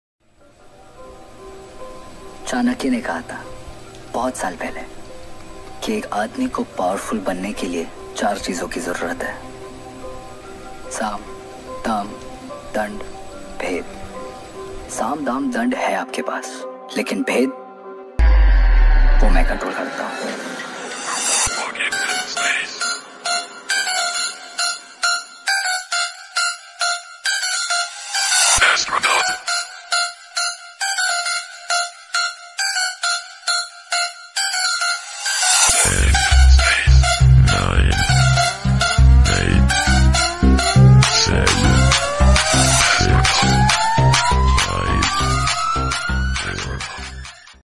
💻 || PC Build || 🛠️ PC Assembled at Home || CPUBuild || 🔥 Short Video
Hello, friends hope you all are healthy as you can see in today's video we have opened our CPU to clean. While I was opening my CPU cabinet for cleaning, I thought why not make a short video, "It's hardware that makes a machine fast. It's software that makes a fast machine slow"  This thing is written in thumbnail because people often open and clean their system to make it fast, but many times they do not know that the hardware and software installed in their system are often they are not coordinated properly because the software they use required hi-performance hardware and they think their computer is slow.
The policy of Saam Daam Dand Bhed is going on since ancient times. Yes, it has been mentioned in Matsya Purana by Lord Matsya. Acharya Chanakya has also discussed these four policies. (साम, दाम, दंड, भेद) in which the meaning of "Bhed" is information. If I explain it in detail, then the "Bhed" means using the personal information of a person and blackmailing it, and using it for your work. A person who has a good knowledge of computer and technical equipment can misuse it this person can either install their own hardware in your computer system or install software and keep spying on your information. Hope you guys have understood the meaning of the dialogue in this video.
Hope you liked this information, if you have any questions or suggestions, please tell me by commenting below.